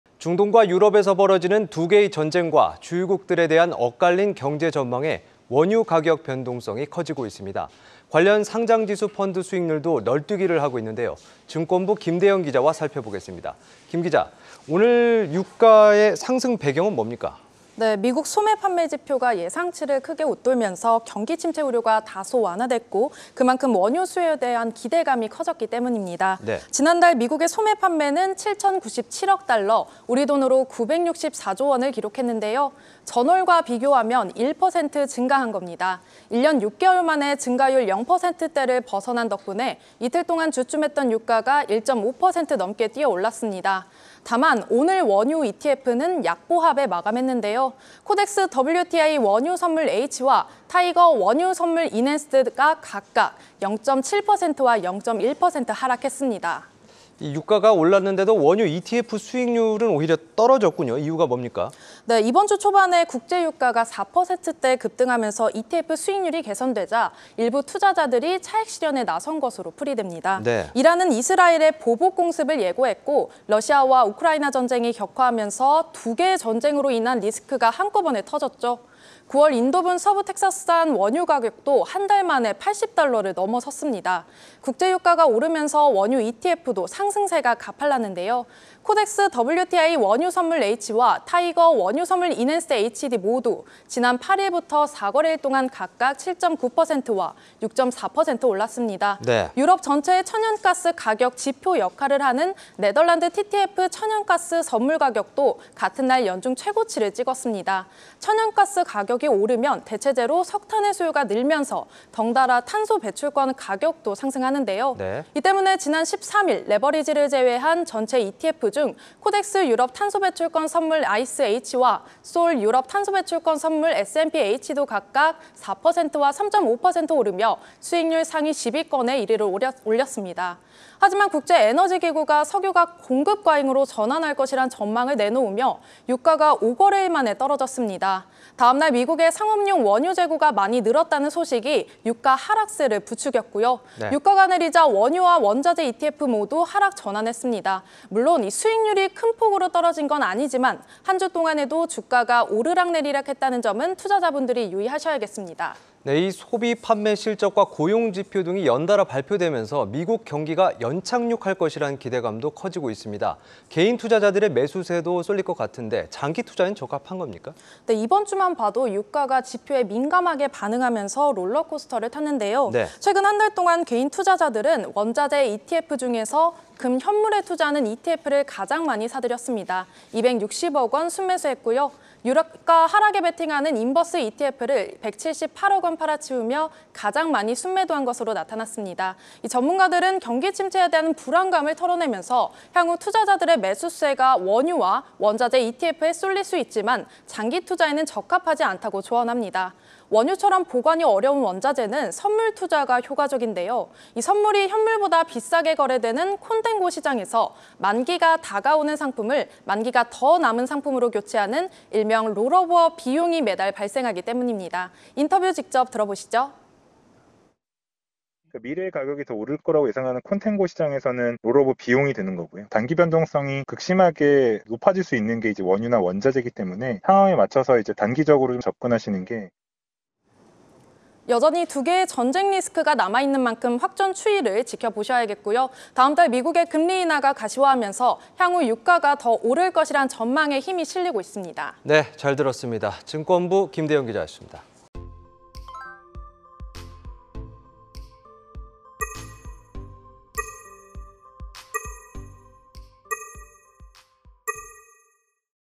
일촉즉발 2개의 전쟁…원유·원자재 ETF '출렁'/한국경제TV뉴스
중동과 유럽에서 벌어지는 2개의 전쟁과 주요국들의에 대한 엇갈린 경제 전망에 원유 가격 변동성이 커지고 있습니다. 관련 상장지수펀드(ETF) 수익률도 널뛰기를 하고 있는데요 증권부 김대연 기자와 살펴보겠습니다.